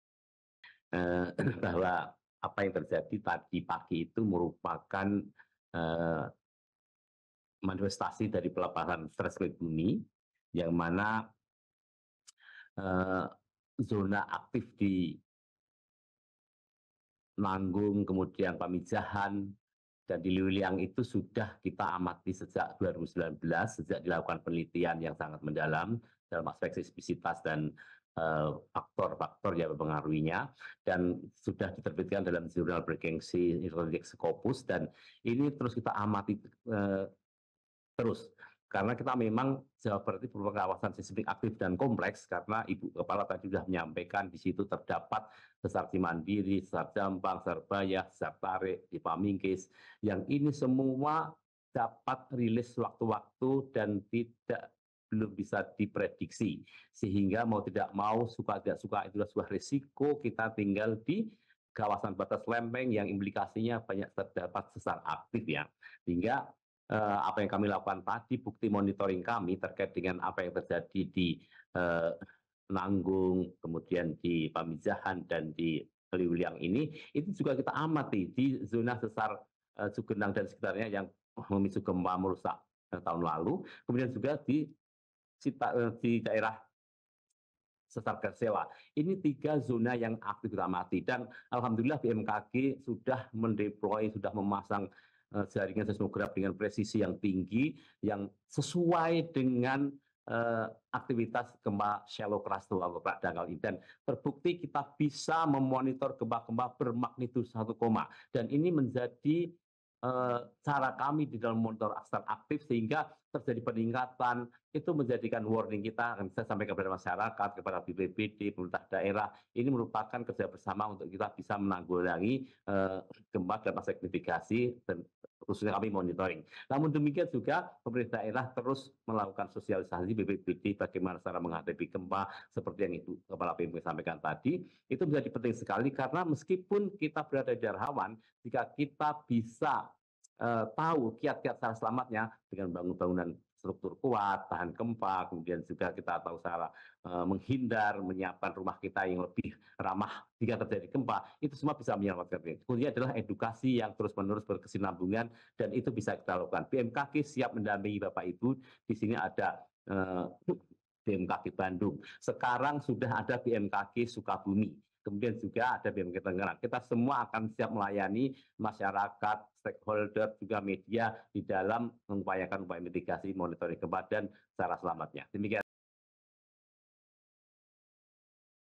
GEMPA BUMI GUNCANG SUKABUMI JAWA BARAT MAGNITUDO 4,7 KEDALAMAN 10 KM
Sejumlah kerusakan bangunan terjadi akibat gempa bumi berkekuatan magnitudo (M) 4,6 yang terjadi pada pukul 06.36 WIB, Kamis (14/12/2023). Wilayah Pamijahan, Kabupaten Sukabumi, Jabar, menjadi wilayah yang paling kuat merasakan getaran gempa bumi. Kedalaman pusat gempa berada di kedalaman 10 kilometer.